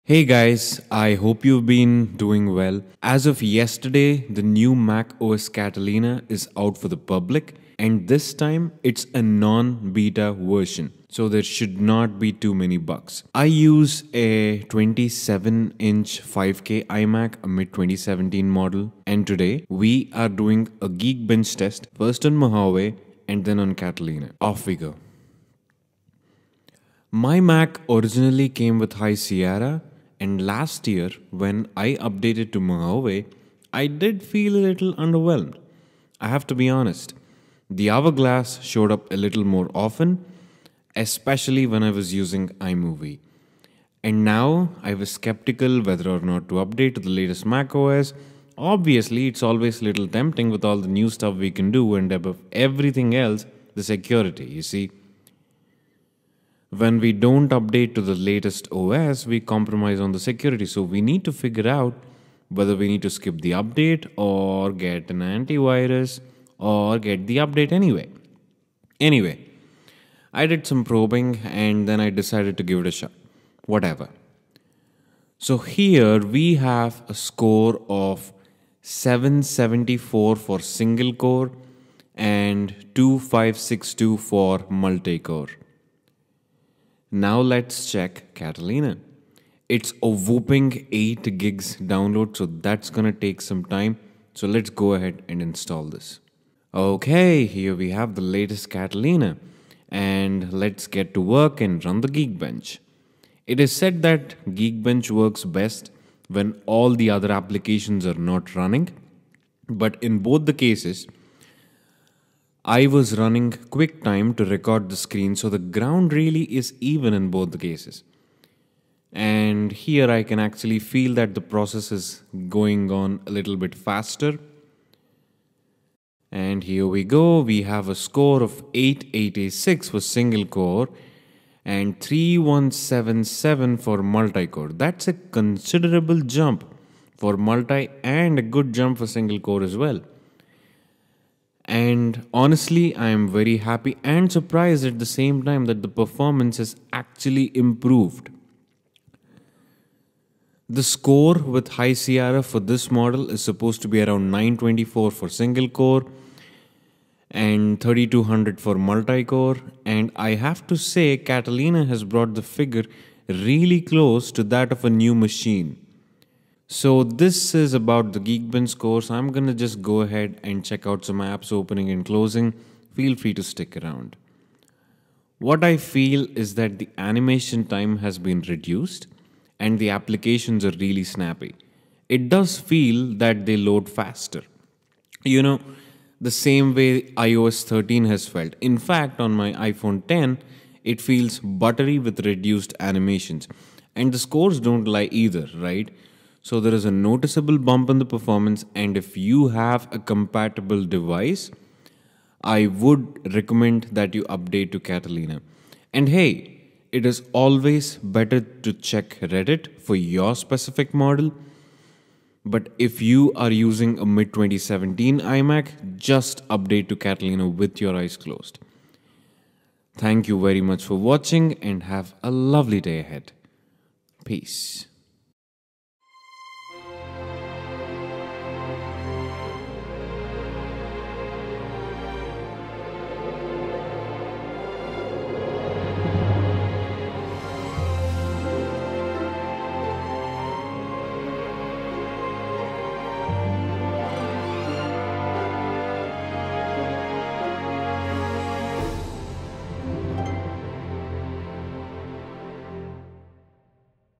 iMac Geekbench Test | Mojave Vs Catalina Performance Test
Is it worth upgrading to Catalina? Is the OS going to make the mid 2017 iMac faster or slow it down? Watch to know.